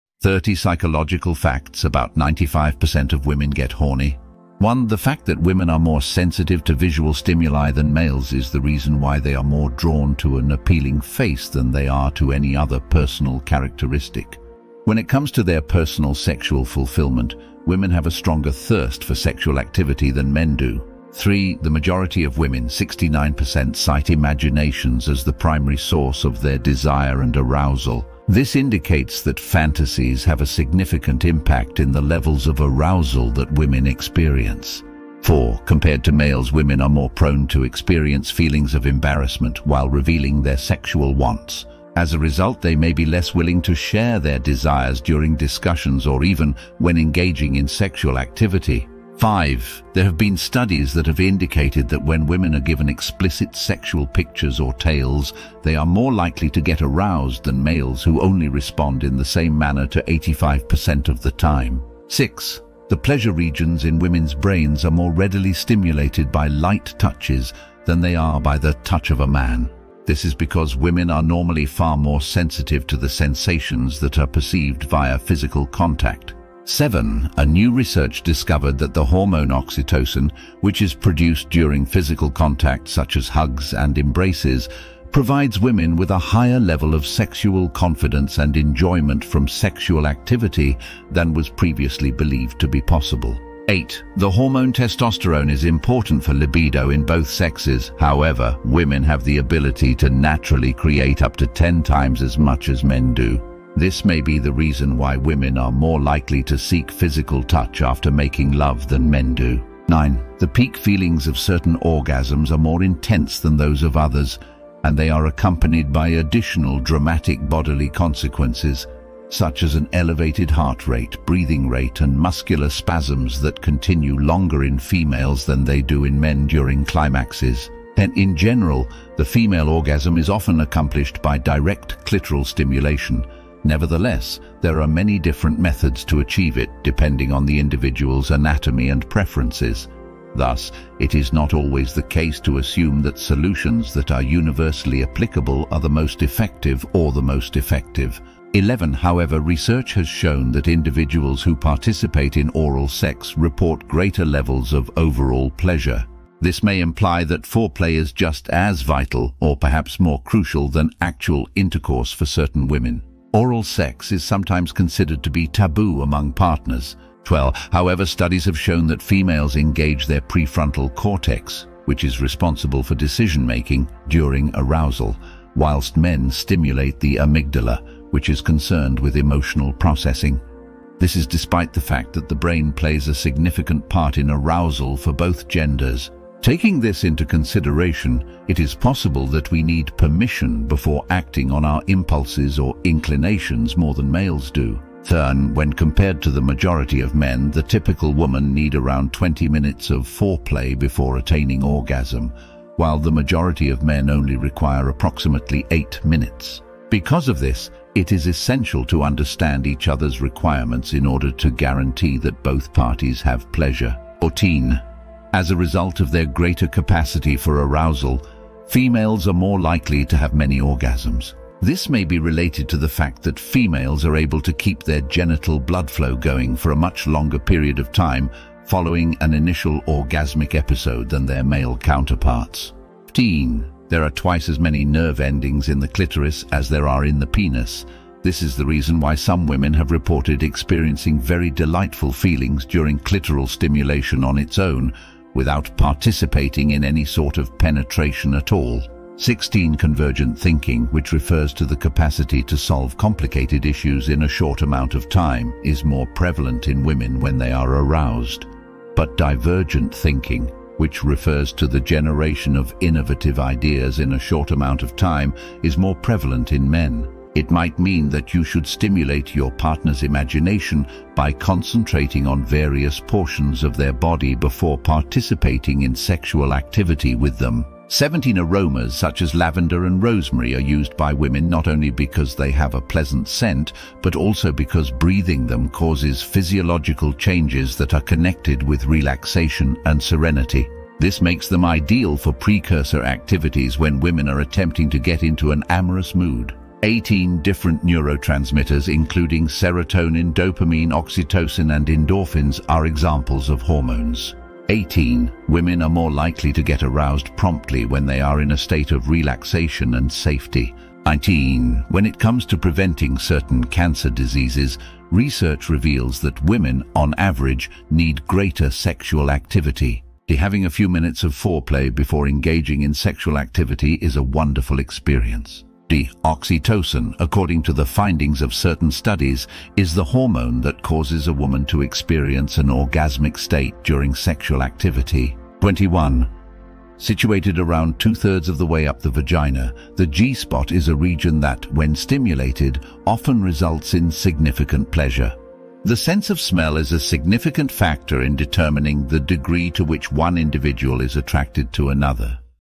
NEW 30 INTERESTING PSYCHOLOGICAL FACTS ABOUT WOMEN: 95% of Women Get H@rny
In this video, we're going to take a look at some interesting psychological facts about women. We'll explore topics like naked attraction, modern wisdom, and more.

These psychological facts about women are fascinating, and they'll help you to better understand women and how they think. If you're looking for insights into modern dating, then this video is for you!

Brian Male